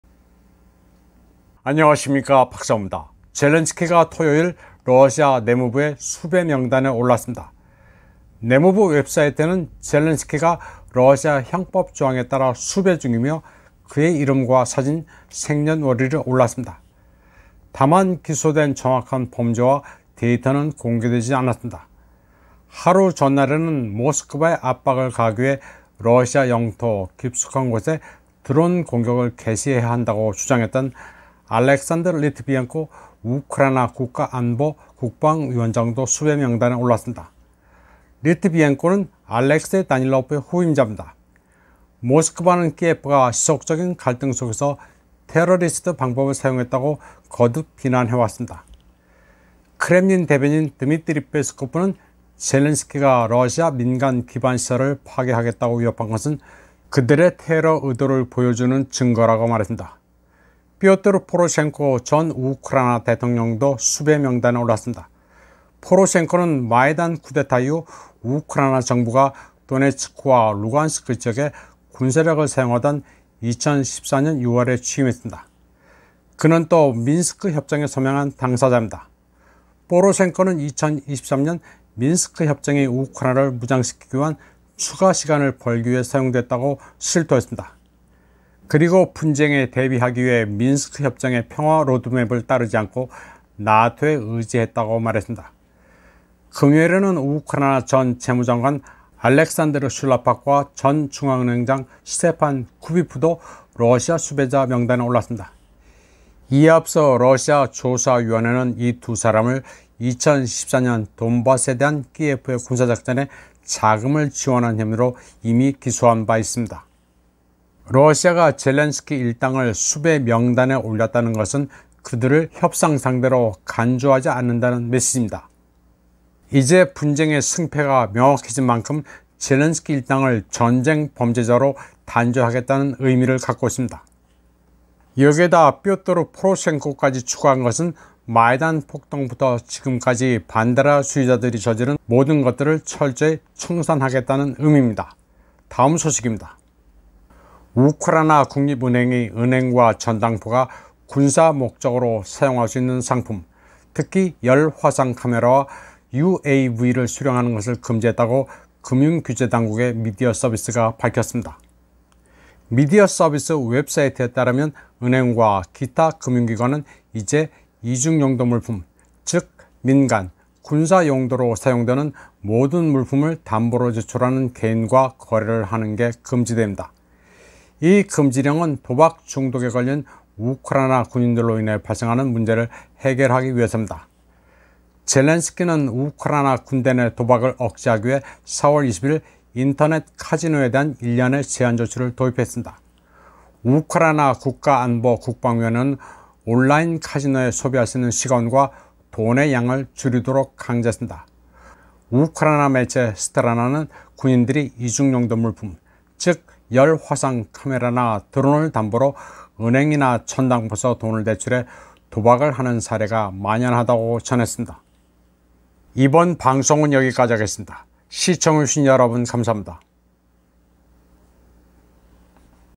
젤렌스키 일당 전범으로 수배!
젤렌스키가 토요일 러시아 내무부의 수배명단에 올랐습니다. 내무부 웹사이트에는 젤렌스키가 러시아 형법조항에 따라 수배중이며 그의 이름과 사진, 생년월일이 올랐습니다. 다만 기소된 정확한 범죄와 데이터는 공개되지 않았습니다.

하루 전날에는 모스크바에 압박을 가하기 위해 러시아 영토 깊숙한 곳에 드론공격을 개시해야 한다고 주장했던 알렉산드르 리트비옌코 우크라이나 국가안보국방위원장도 수배명단에 올랐습니다. 리트비옌코는 알렉세이 다닐로프의 후임자입니다. 모스크바는 키예프가 지속적인 갈등속에서 테러리스트 방법을 사용했다고 거듭 비난해왔습니다. 크렘린 대변인 드미트리 페스코프는 젤렌스키가 러시아의 민간 기반 시설을 파괴하겠다고 위협한 것은 그들의 테러 의도를 보여주는 증거라고 말했습니다.

표트르 포로셴코 전 우크라이나 대통령도수배 명단에 올랐습니다. 포로셴코는 마이단 쿠데타 이후 우크라이나 정부가 도네츠크와 루간스크 지역에 군사력을 사용하던 2014년 6월에 취임했습니다. 그는 또 민스크 협정에 서명한 당사자입니다. 포로셴코는 2023년 민스크 협정이 우크라이나를 무장시키기 위한 추가 시간을 벌기 위해 사용되었다고 실토했습니다. 그리고 분쟁에 대비하기 위해 민스크 협정의 평화 로드맵을 따르지 않고 NATO에 의지했다고 말했습니다.

금요일에는 우크라이나 전 재무장관 알렉산드르 슐라팍과 전 중앙은행장 스테판 쿠비프도 러시아 수배자 명단에 올랐습다. 이에 앞서 러시아 조사위원회는 이 두사람을 2014년 돈바스에 대한 키예프의 군사 작전에 자금을 지원한 혐의로 이미 기소한 바 있습니다.

러시아가 젤렌스키 일당을 수배명단에 올렸다는 것은 그들을 협상상대로 간주하지 않는다는 메시지입니다. 이제 분쟁의 승패가 명확해진 만큼 젤렌스키 일당을 전쟁범죄자로 단죄하겠다는 의미를 갖고 있습니다. 여기에다 표트르 포로셴코까지 추가한 것은 마이단 폭동부터 지금까지 반데라주의자들이 저지른 모든 것들을 철저히 청산하겠다는 의미입니다.

우크라이나 국립은행이 은행과 전당포가 군사 목적으로 사용할 수 있는 상품, 특히 열화상 카메라와 UAV를 수령하는 것을 금지했다고 금융 규제 당국의 미디어서비스가 밝혔습니다. 미디어서비스 웹사이트에 따르면 은행과 기타 금융 기관은 이제 이중용도 물품, 즉 민간, 군사용도로 사용되는 모든 물품을 담보로 제출하는 개인과 거래를 하는 게 금지됩니다.

이 금지령은 도박 중독에 걸린 우크라이나 군인들로 인해 발생하는 문제를 해결하기 위해서입니다. 젤렌스키는 우크라이나 군대 내 도박을 억제하기 위해 4월 20일 인터넷 카지노에 대한 일련의 제한 조치를 도입했습니다. 우크라이나 국가안보국방위원회는 온라인 카지노에 소비할 수 있는 시간과 돈의 양을 줄이도록 강제했습니다. 우크라이나 매체 Strana는 군인들이 이중용도 물품, 즉 열화상 카메라나 드론을 담보로 은행이나 전당포에서 돈을 대출해 도박을 하는 사례가 만연하다고 전했습니다.

*자발적 후원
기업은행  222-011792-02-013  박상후